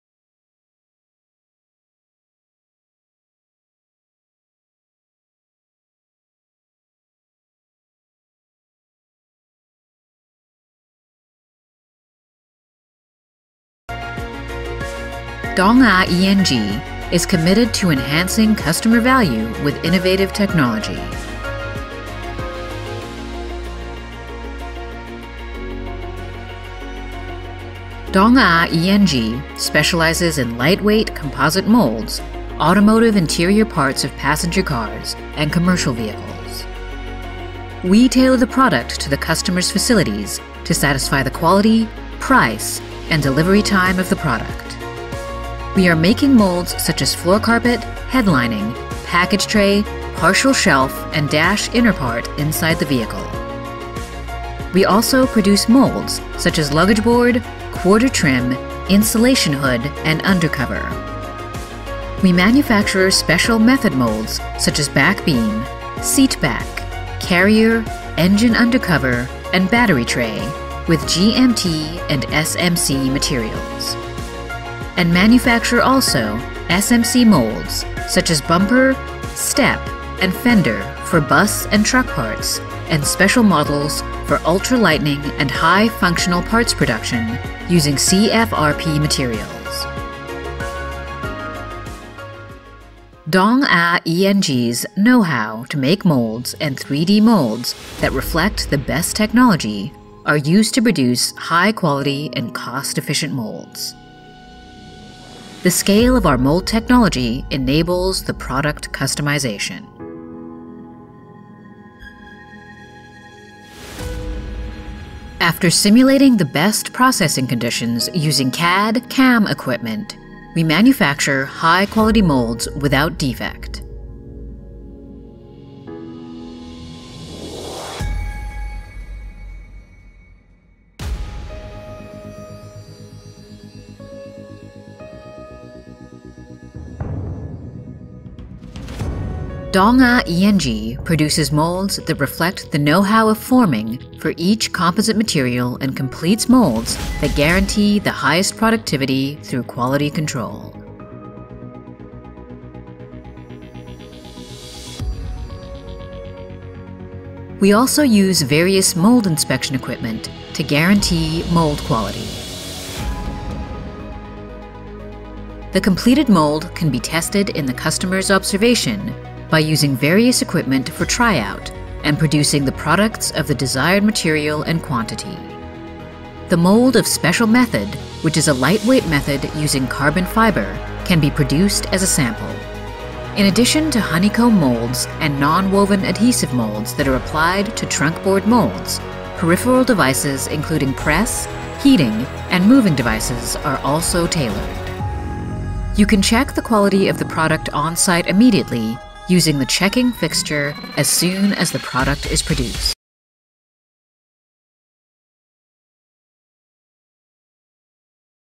Automotive Lightweight Composite Interior Molds
Automotive lightweight composite interior trim molds for headliners, floor carpets, wheel arch liners, door trim, package tray, trunk lid, engine cover, dash inner & outer, underbody, luggage trim, and hood insulators.

Special method molds for ultralight and high functioning pats production, such as engine cover, battery tray, seat back, and back beam.

LPR Global Inc.